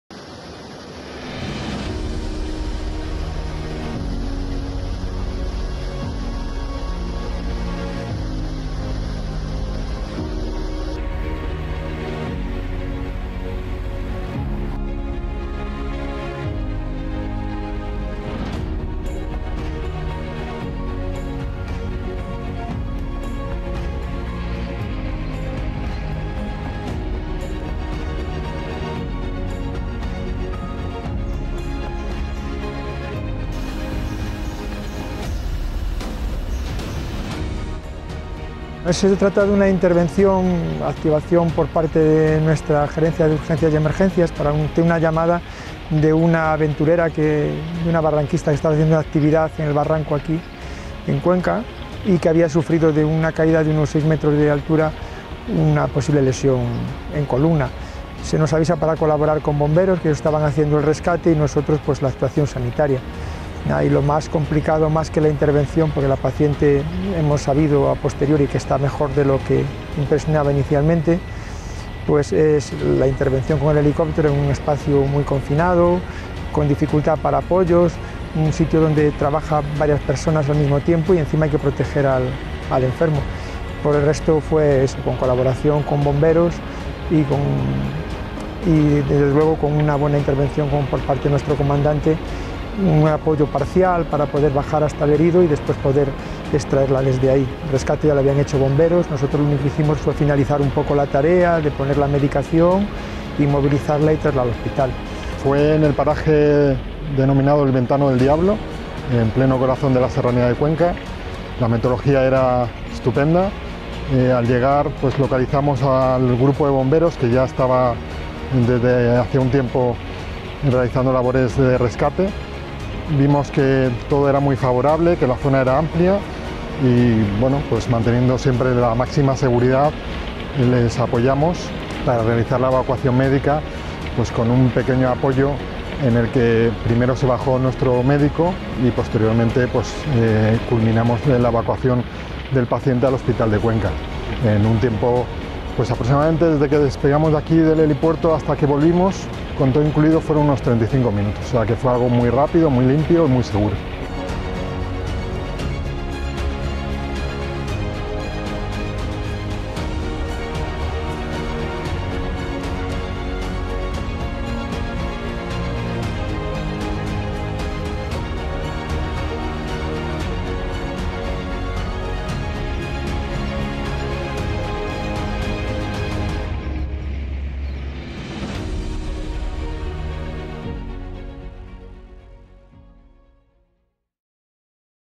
Rescatada persona cerca del Ventano del Diablo (Cuenca)
Una persona accidentada ha sido rescatada este domingo cerca del Ventano del Diablo, en Villalba de la Sierra (Cuenca), y trasladada al hospital Virgen de la Luz de Cuenca.

(Fuente: Gobierno Castilla-La Mancha)